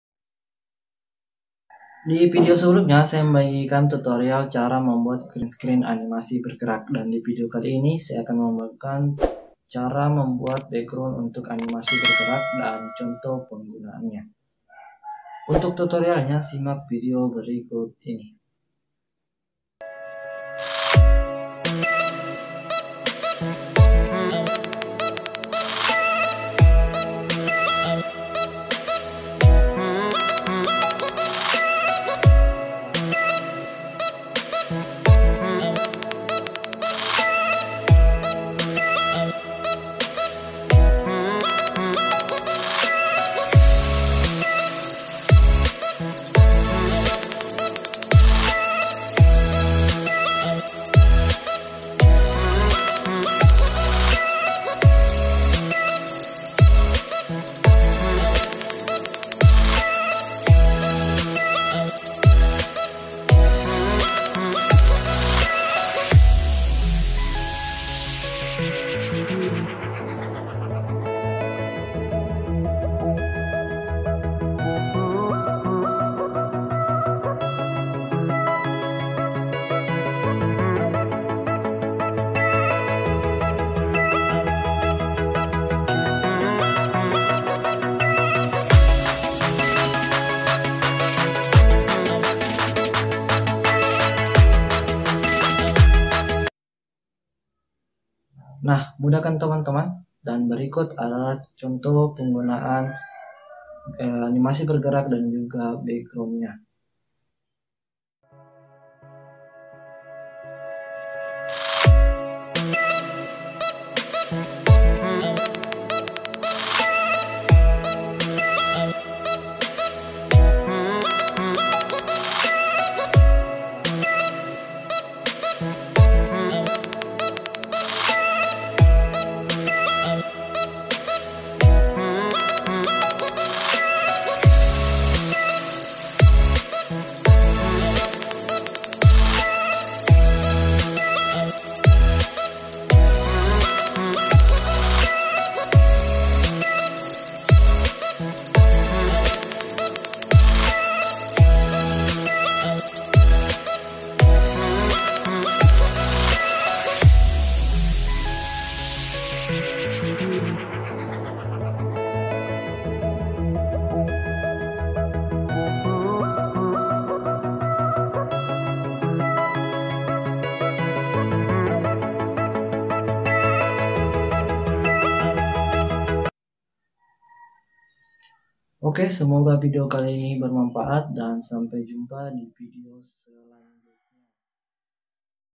Cara membuat background animasi bergerak dan contoh penggunaannya
Video ini lanjutan video sebelumnya. Divideo sebelumnya Cara membuat animasi bergerak. cek video itu agar conek yah

Semoga video kali ini bermanfaat.